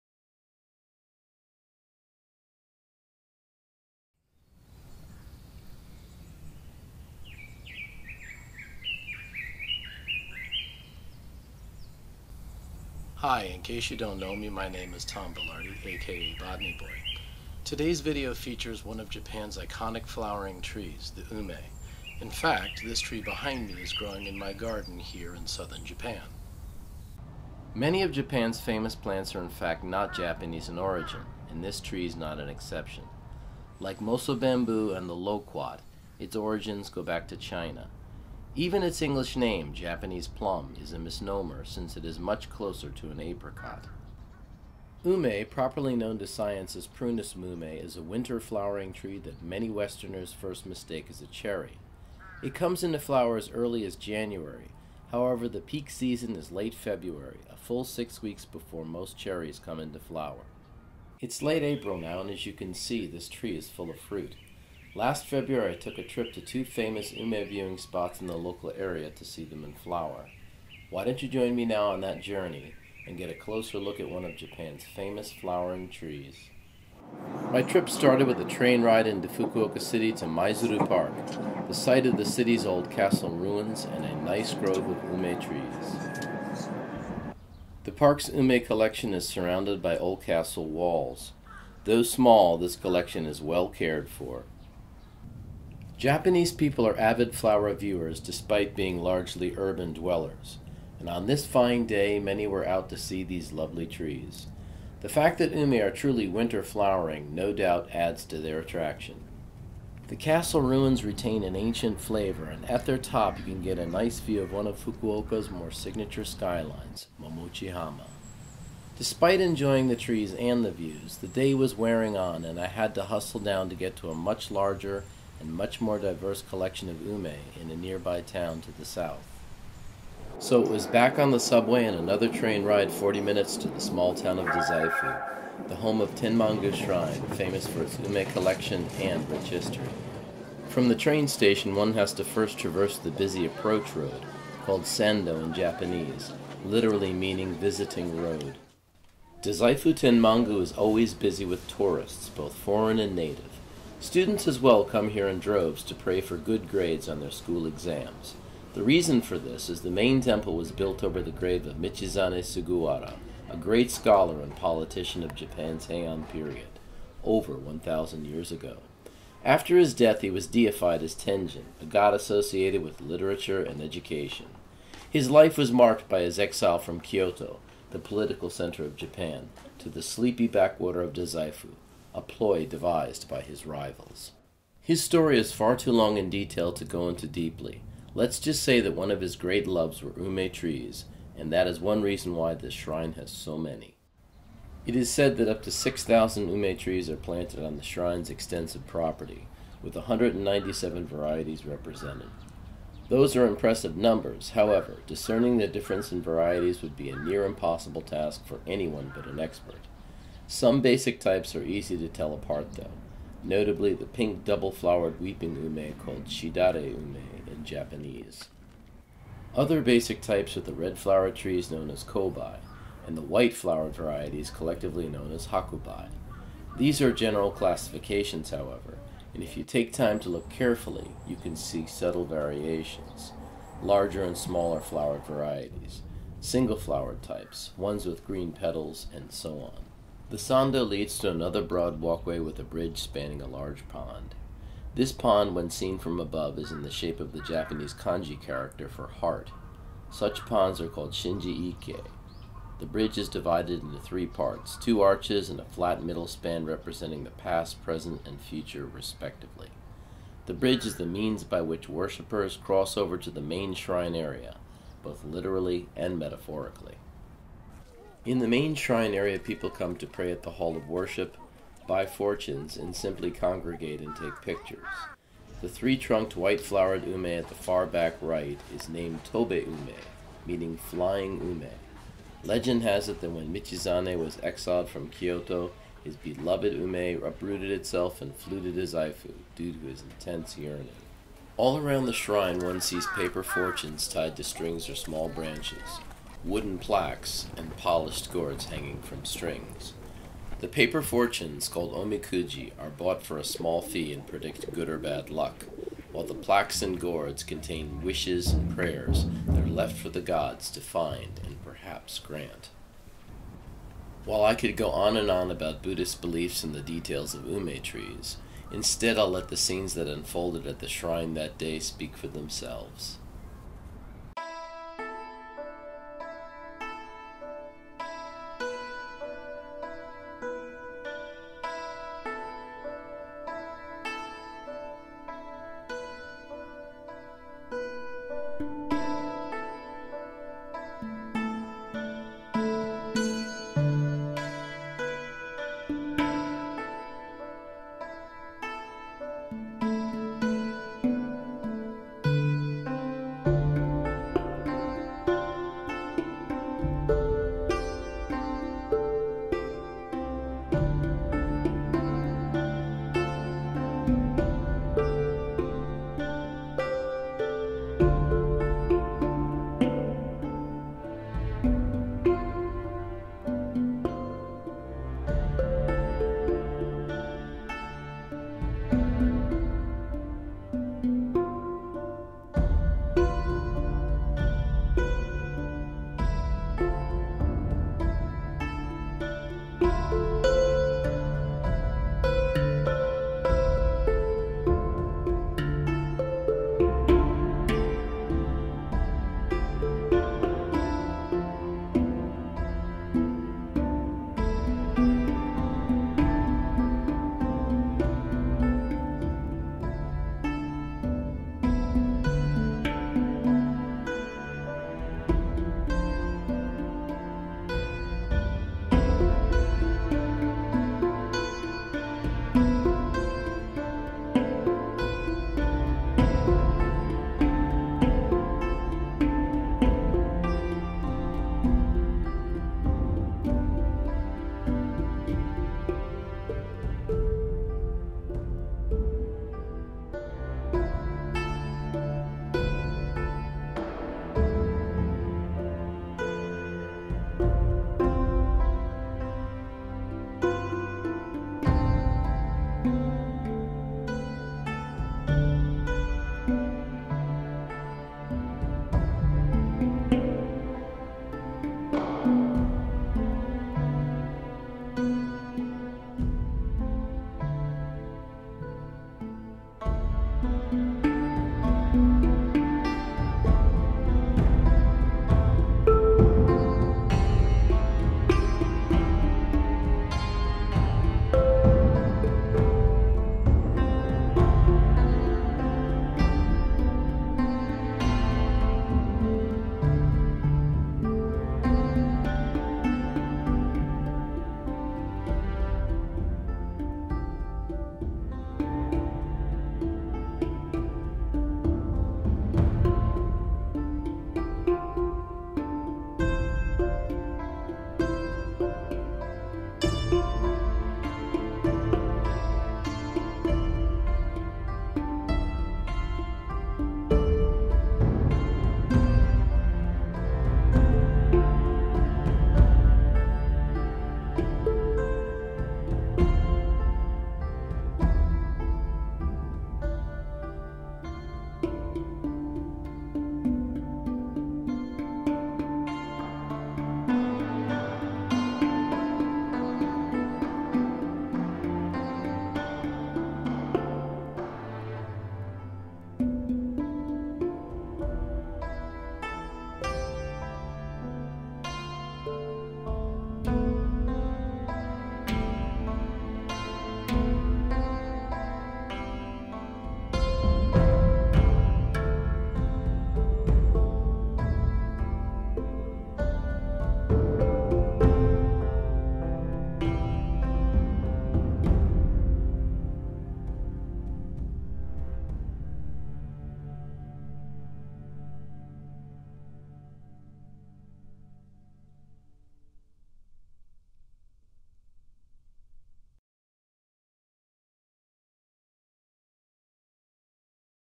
Ume - Japanese Flowering Plum Trees
Late February is the blooming time for one of Japan’s iconic trees, ume, the Japanese flowering plum.  In this video we’ll visit two ume viewing spots - the old castle ruins in Maizuru Park in Fukuoka City and Tenmangu Shrine in Dazaifu City.  Join the tour of these two historic sites and the winter flowering trees that adorn them.